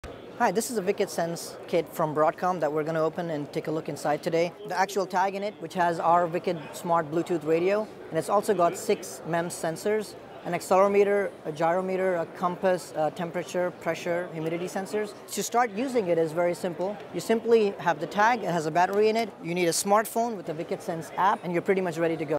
See what is included in a WICED Sense Kit
See how the WICED sense kit offers, and the cool details it includes to connect to your cell phone.  The WICED sense kit includes;  bluetooth radio, compass, gyro meter, temperature, pressure meter and more.